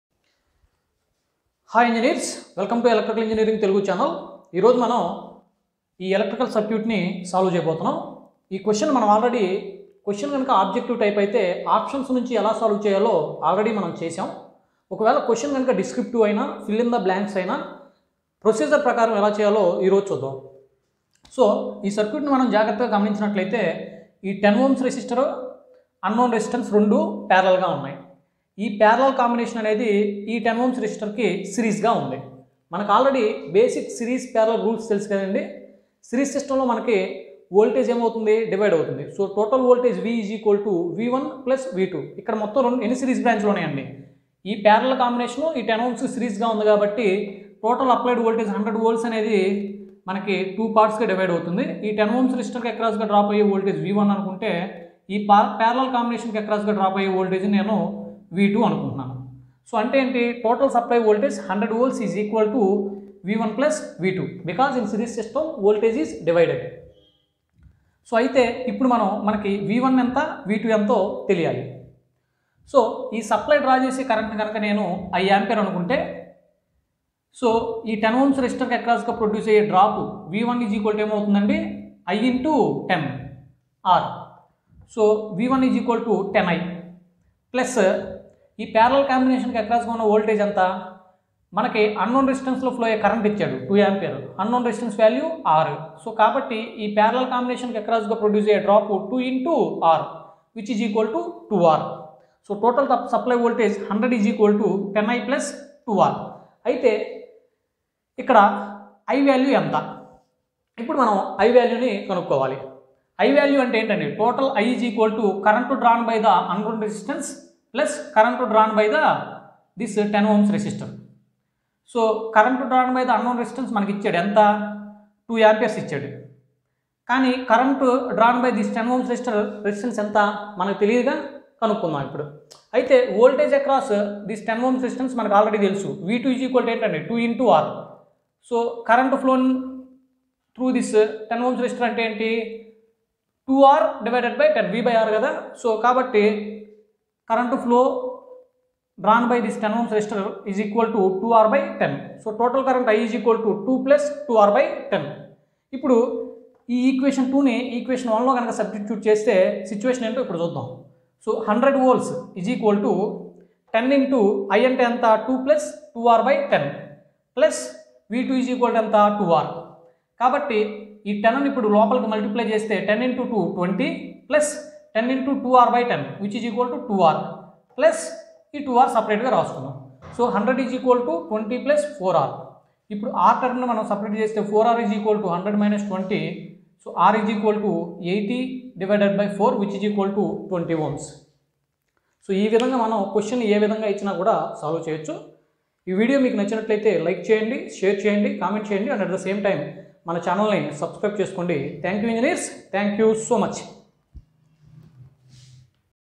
Series and parallel circuits. Previous gate question
Simple Series and parallel circuits.  Previous gate question . step by step procedure.

Electrical circuits in telugu